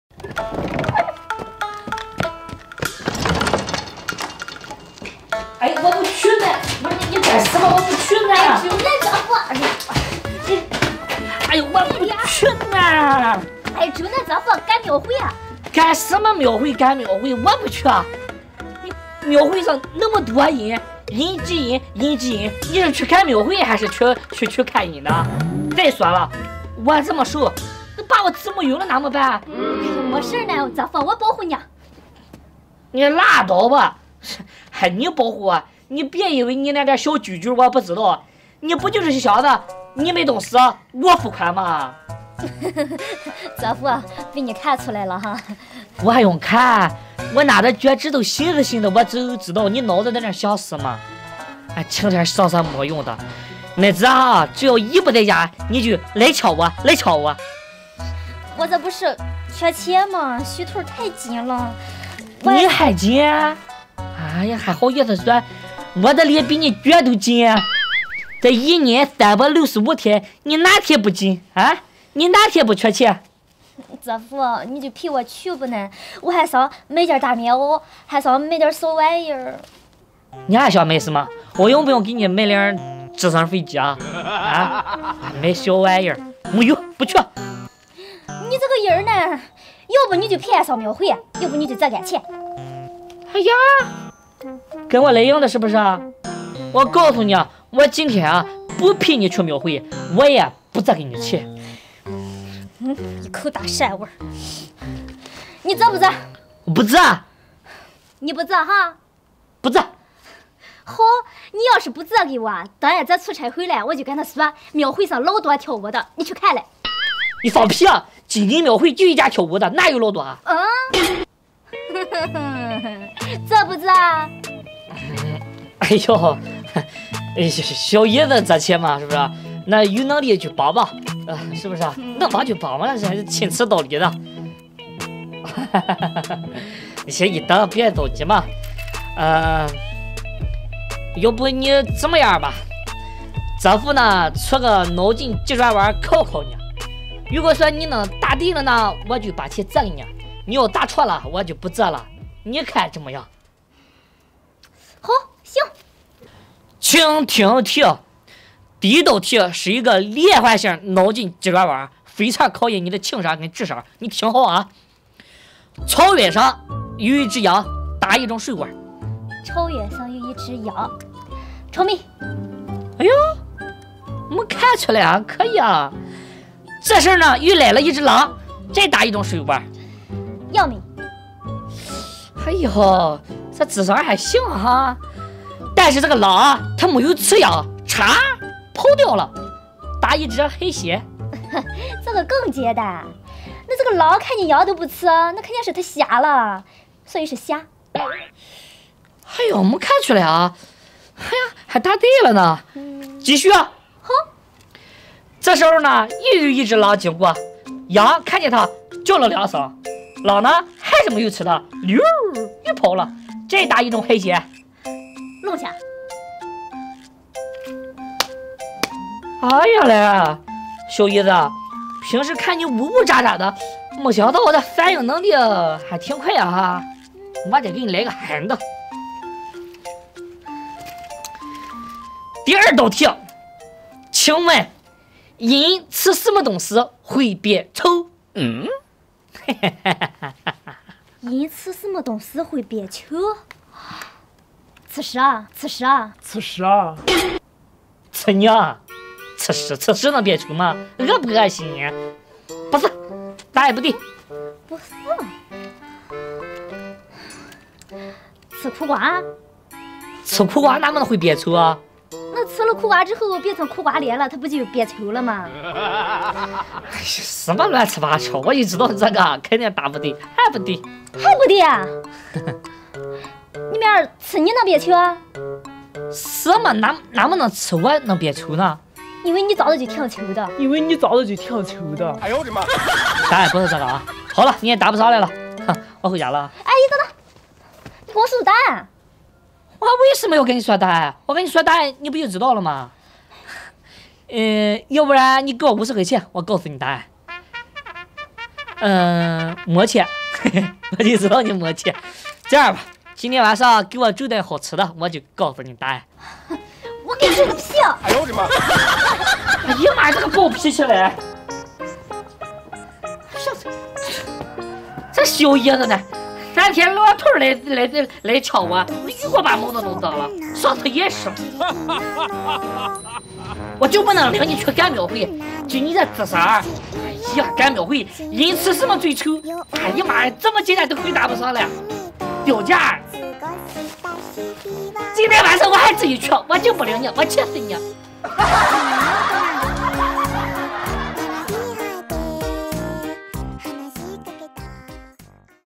哥哥问妹妹，吃什么东西会变丑，不料妹妹的回答太有才了【乡村喜剧人】
搞笑有趣的情感主播改行演喜剧，会带来什么不一样的惊喜呢？

大家好，我是乡村喜剧人，一个可以给你们带来开心的地方~